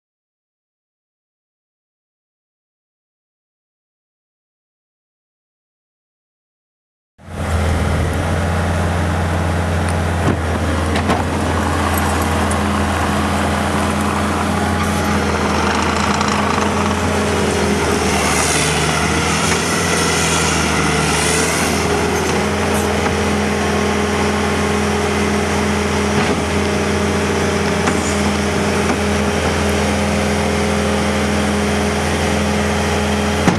2011 Bobcat T750 skid steer for sale | sold at auction December 30, 2015
2011 Bobcat T750 skid steer
3,946 hours on meter
Four cylinder diesel engine
Hydrostatic transmission
Enclosed cab
AC and heat
AM/FM
Wipers
80"W bucket
Bolt-on teeth
Hydraulic quick coupler
Auxiliary hydraulics
Auxiliary electrical outlet
17"W tracks
Updated cooling fan
10,327 lbs. base operating weight
Parts manual
Serial ANKA11276

It was located in Independence, MO, and we sold it to the highest bidder for $26,950.